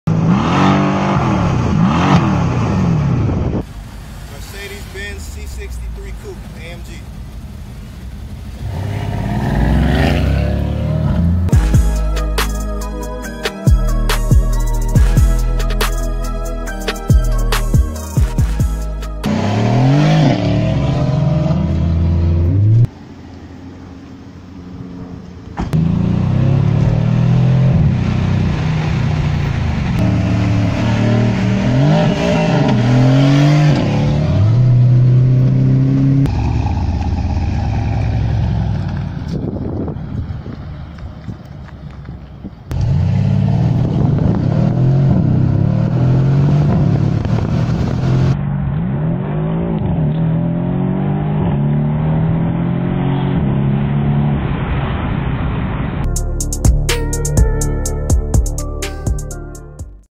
2017 Mercedes-Benz C63 AMG - "Promotional Video"
Mercedes Benz C63 is a german Luxury performance coupe with power, road manners, comfort and creature features. Huge moonroof, exhaust cutouts, huge brake discs and calipers for great stopping power. 4 Liter twin turbo V8, with instant start and shut off to save you fuel.

Mercedes-Benz C63 is rear wheel drive but has great stability unlike certain other cars we know of such as mustangs. very agile vehicle and the turning is sharp. Even though it's a coup, it is spacious for both the driver and passenger. the audio system,  Navigational, & safety features are second to none!